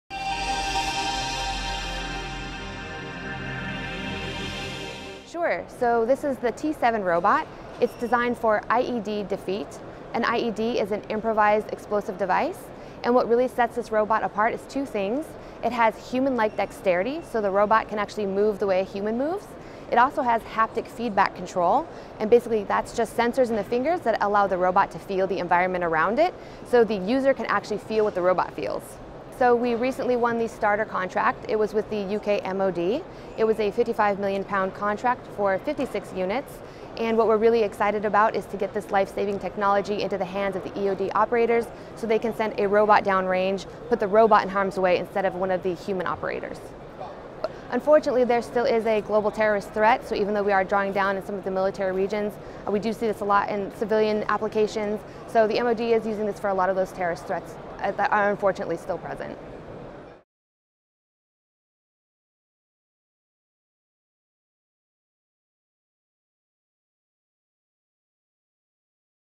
Harris Reveals Robot Technology
At DSEI 2017, the UK MoD awarded Harris Corporation a contract worth up to £55 million ($70 million) to provide its T7 Robotic system to support UK explosive ordnance disposal missions.

Shephard spoke with Harris about the technology and the details of the recent contract award.

Read more news coverage of DSEI from the show's Official Digital Media Partner, Shephard Media